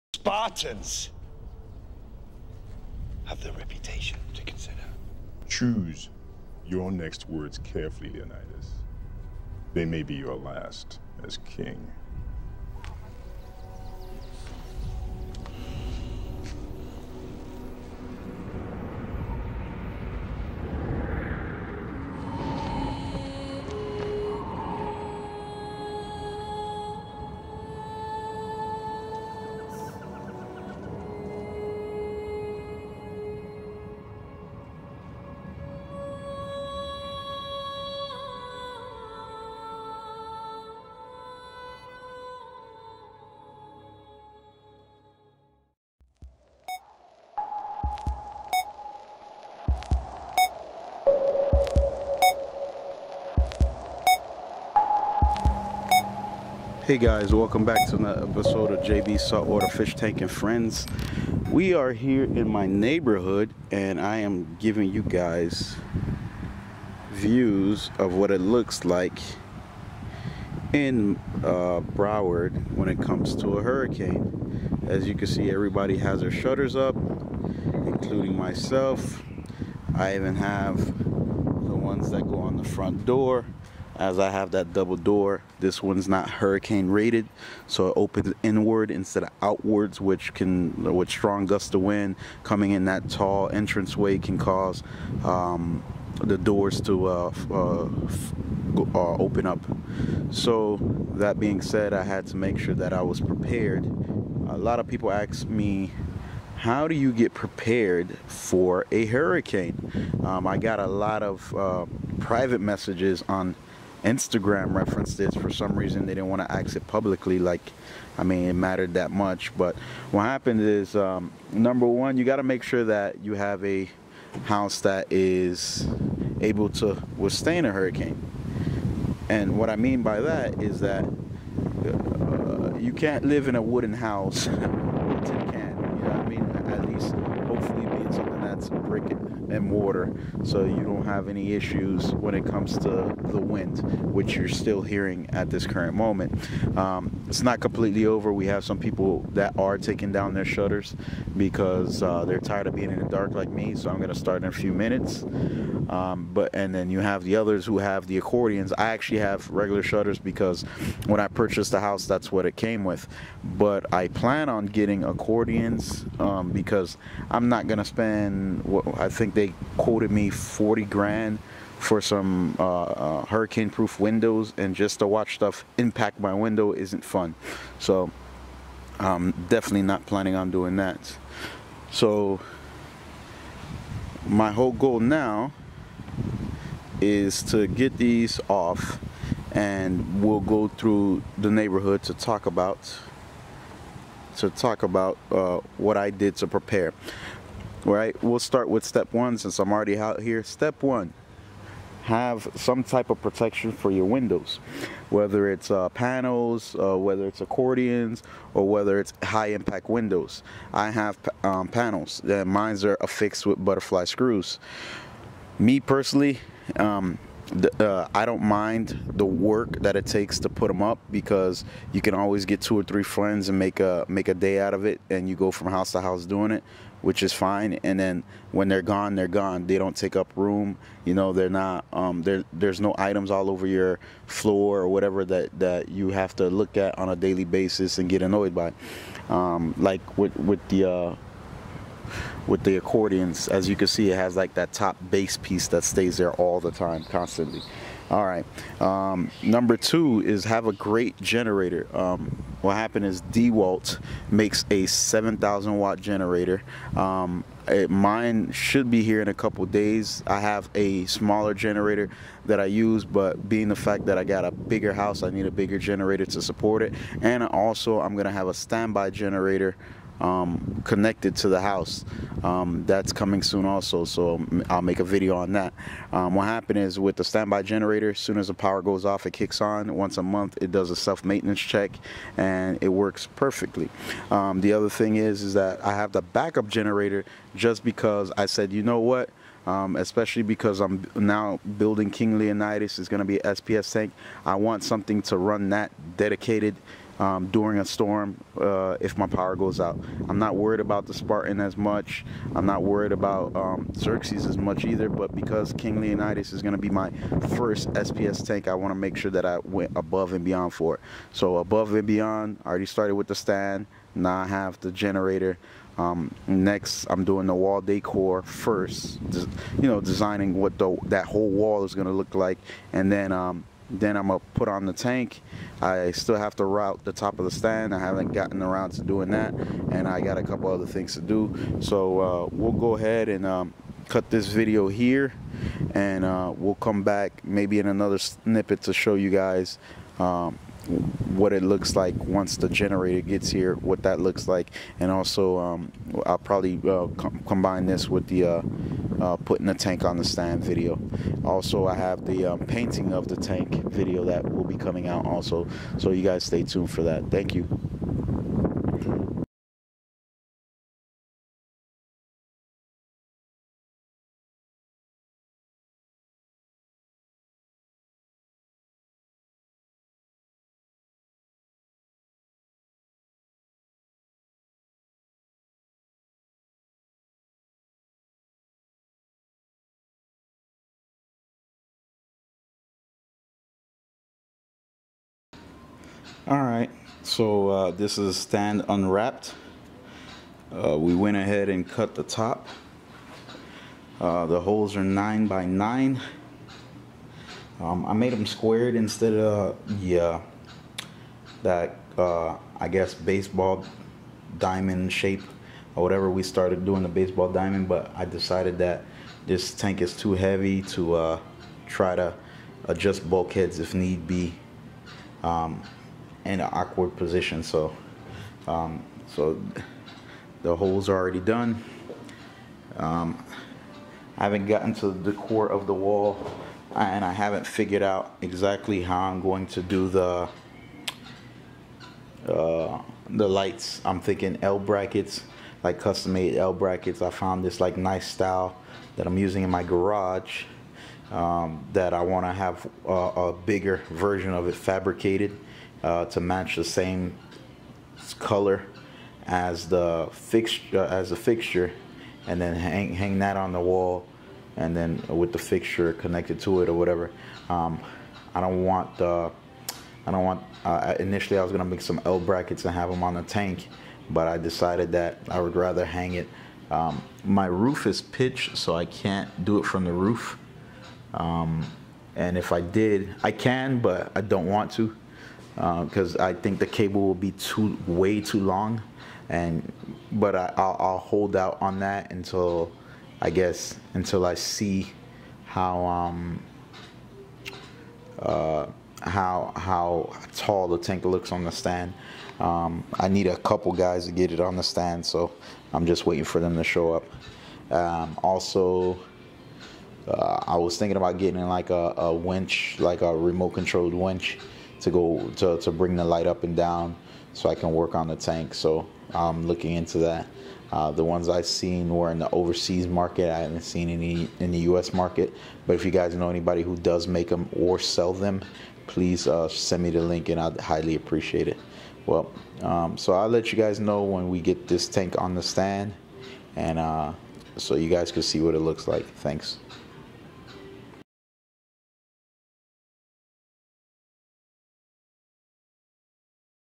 "King Leonidas" The Reef Tank | Episode 2 | Hurricane Matthew & Working on Stand
On episode 2 for this 300g Deep Dimension S.P.S. Dominated Reef in beautiful Pembroke Pines, Florida. I wanted to talk about preparing for a hurricane and some of the methods I use to do it. My idea of what the holes in the stand should be and the wall prep prior to tank placement.